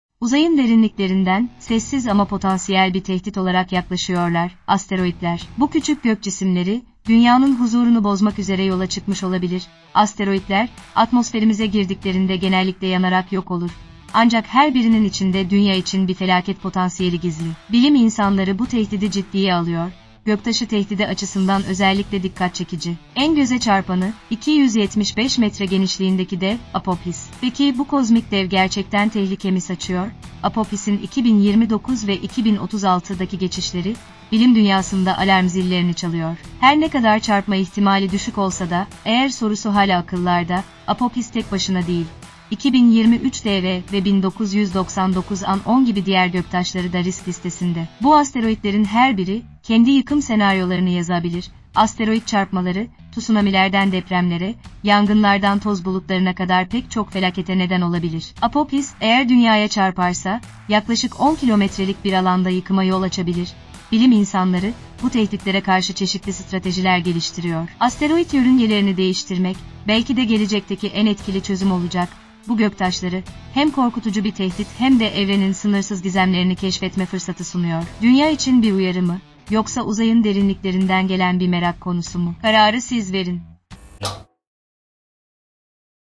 Gerçek Hayatta Armageddon: Dünya Yok Oluşun Eşiğinde mi?
"Uzayın derinliklerinden, sessiz ama potansiyel bir tehdit olarak yaklaşıyorlar: Asteroitler. Bu küçük gök cisimleri, Dünya'nın huzurunu bozmak üzere yola çıkmış olabilir."

"Asteroitler, atmosferimize girdiklerinde genellikle yanarak yok olur, ancak her birinin içinde Dünya için bir felaket potansiyeli gizli. Bilim insanları bu tehdidi ciddiye alıyor."

"Göktaşı tehdidi açısından özellikle dikkat çekici. En göze çarpanı, 275 metre genişliğindeki dev, Apophis. Peki, bu kozmik dev gerçekten tehlike mi saçıyor?"

"Apophis'in 2029 ve 2036'daki geçişleri, bilim dünyasında alarm zillerini çalıyor. Her ne kadar çarpma ihtimali düşük olsa da, 'eğer' sorusu hala akıllarda."

"Apophis tek başına değil; 2023 DW ve 1999 AN10 gibi diğer göktaşları da risk listesinde. Bu asteroitlerin her biri, kendi yıkım senaryolarını yazabilir."

"Asteroit çarpmaları, tsunamilerden depremlere, yangınlardan toz bulutlarına kadar pek çok felakete neden olabilir. Apophis, eğer Dünya'ya çarparsa, yaklaşık 10 kilometrelik bir alanda yıkıma yol açabilir."

"Bilim insanları, bu tehditlere karşı çeşitli stratejiler geliştiriyor. Asteroit yörüngelerini değiştirmek, belki de gelecekteki en etkili çözüm olacak."

"Bu göktaşları, hem korkutucu bir tehdit hem de evrenin sınırsız gizemlerini keşfetme fırsatı sunuyor. Dünya için bir uyarı mı, yoksa uzayın derinliklerinden gelen bir merak konusu mu? Kararı siz verin."